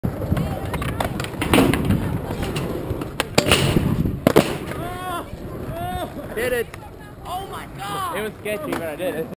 Crook Grind Nollie Heelflip Out
Here's my second crook nollie heelflip out I landed. Its sketchy with the toe drag but I wanted to upload it anyways because I was hyped on it! The first one I landed was perfect tho!